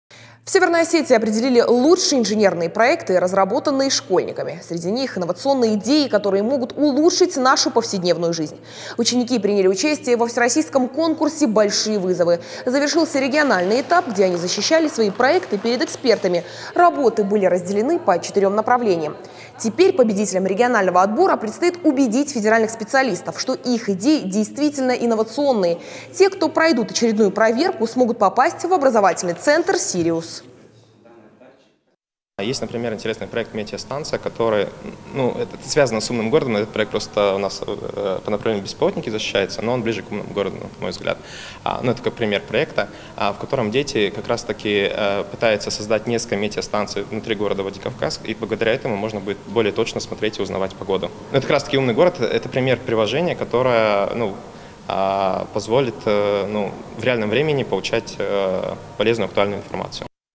В Северной Осетии завершился региональный этап Всероссийского конкурса «Большие вызовы»
В Северной Осетии определили лучшие инженерные проекты, разработанные школьниками. Среди них инновационные идеи, которые могут улучшить нашу повседневную жизнь.

Ученики приняли участие во Всероссийском конкурсе «Большие вызовы». Завершился региональный этап, где они защищали свои проекты перед экспертами. Работы были разделены по 4 направлениям. Теперь, победителям регионального отбора предстоит убедить федеральных специалистов, что их идеи - действительно инновационные. Те, кто пройдут очередную проверку, смогут попасть в образовательный центр «Сириус».
Мы рядом в:
ok.ru/irystontv